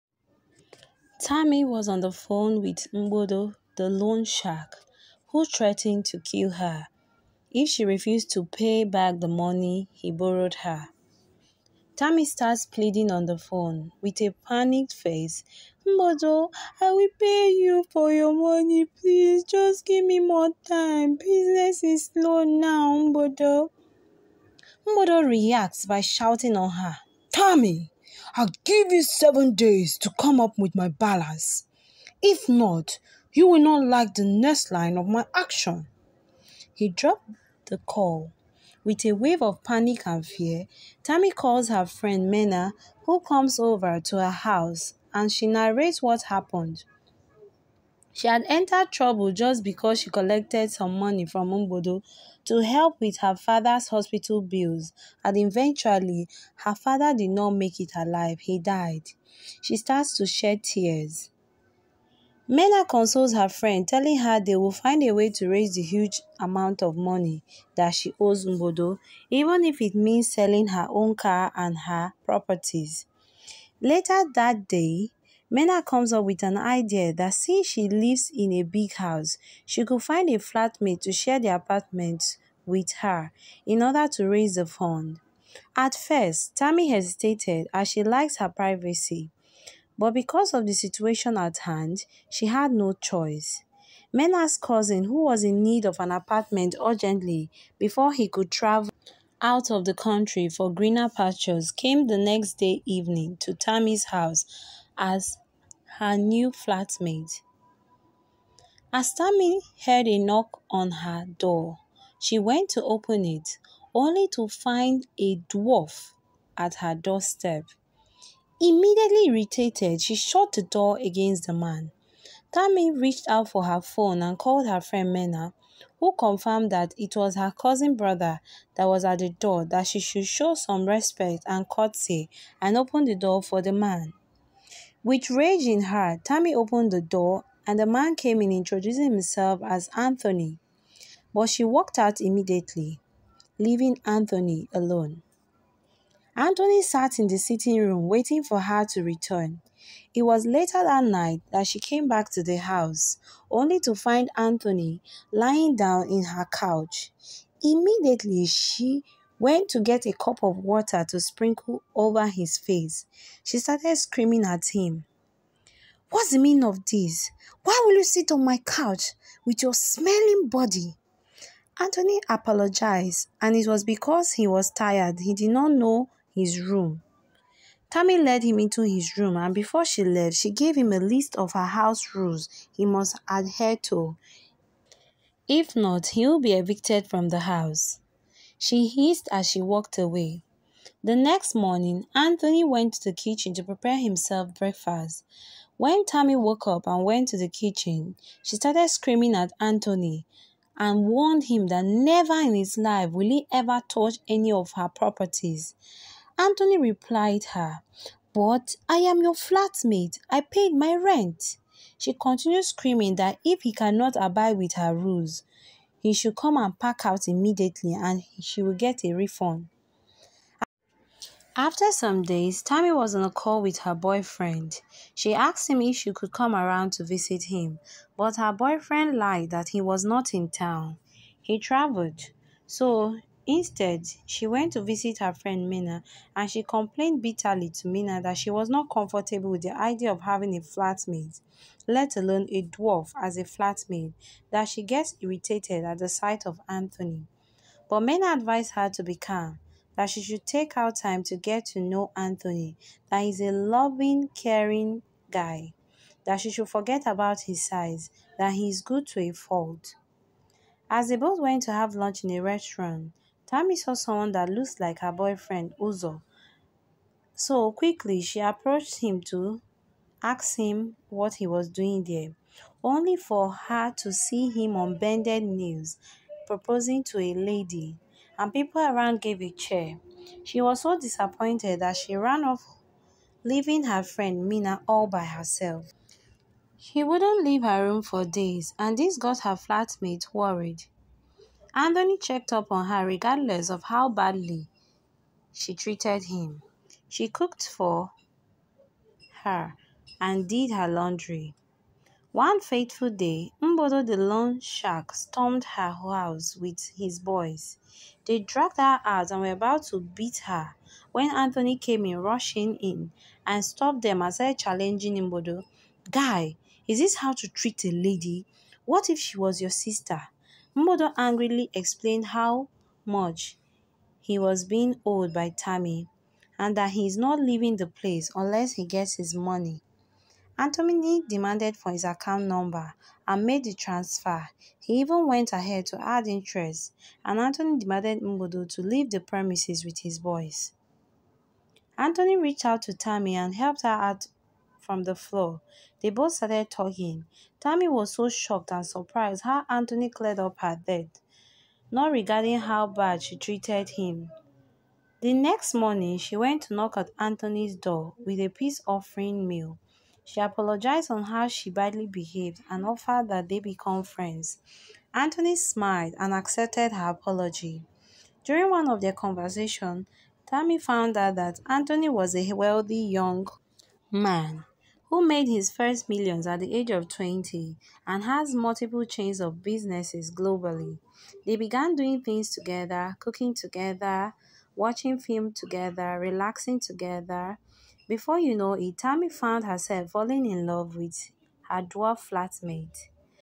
My dwarf roommate i despised later became the love of my life#africanfolktales
Tammy falls in love with her dwarf flatmate she so despised because of his size.  He turned out to be the love of her life she could not trade for anything in this world